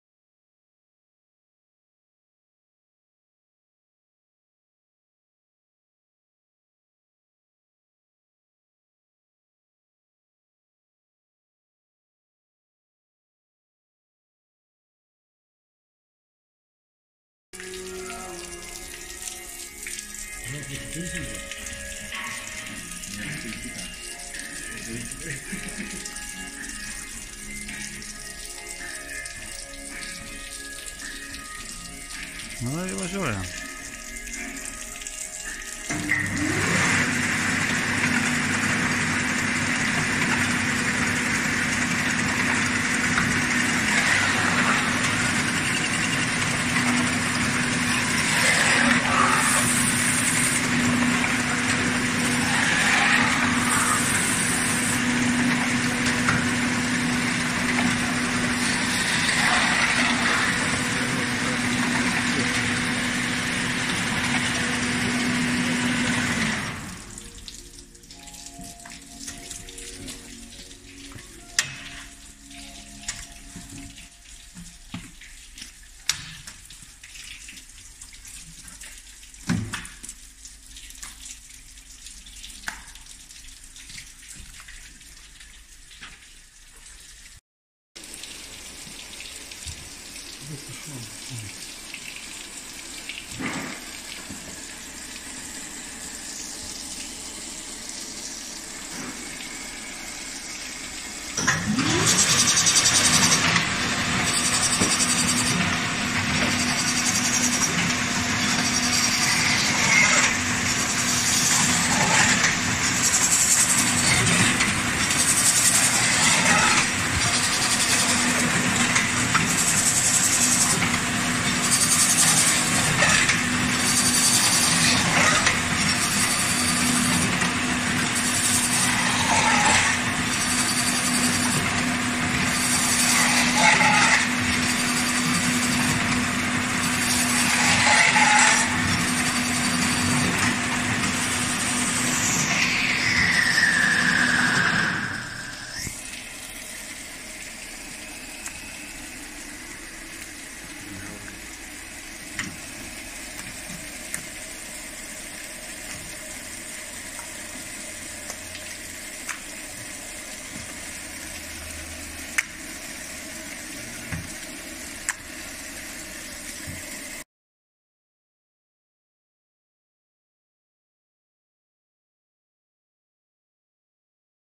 How to efficiently fillet a cod on Baader 189
Need to increase capacity at your fish workshop? We know how! Save your time and money, just make a request for this machine.